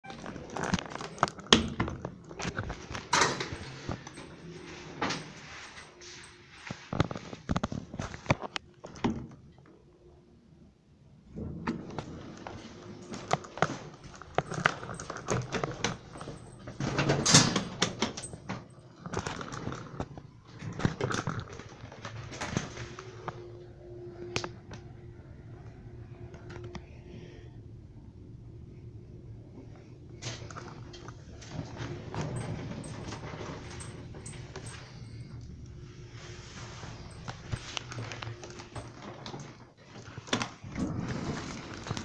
三菱エレベーター
宇多津グランドホテルにあります。まだ未更新のままで運転しています。機種はアクセルシリーズです。
メーカー:三菱
設置年:1989年
定員:13名
積載:900kg
キャンセル機能:なし
アナウンス:なし
戸閉ブザー:なし
到着ベル音:なし
ボタン確認音:なし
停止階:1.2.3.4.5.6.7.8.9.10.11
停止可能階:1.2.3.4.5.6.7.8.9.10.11